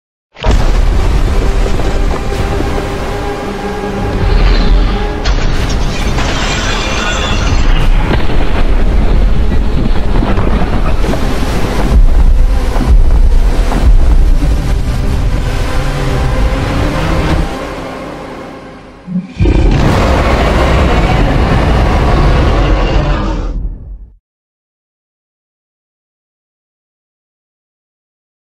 Overmoth 4.0 Moveset suggestion Teaser
Of course im doing this one after the Alzaro one.

I actually have some really good ideas for him.

Some people who commented in the last prediction video on why I’m not just putting these in the Pk discord, it’s because I don’t have access to the community servers, or have the proper roles.

But a last, we will just have to see.